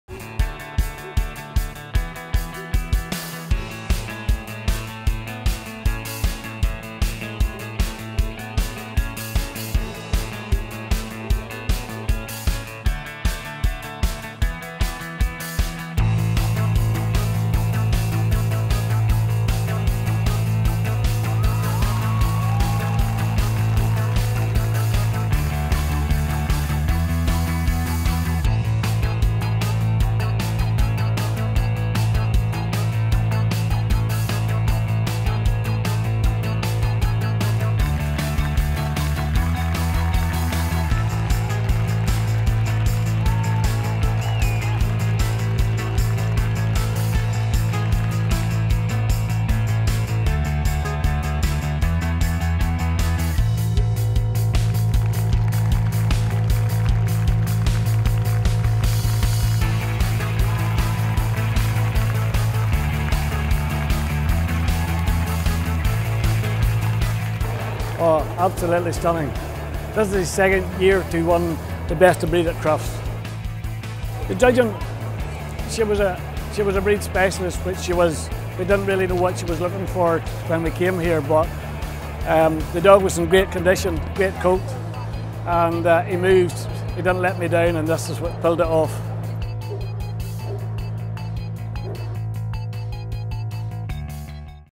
dfs Crufts 2011 - Best of Breed Newfoundland
Best of Breed competition for the Newfoundland at this years dfs Crufts.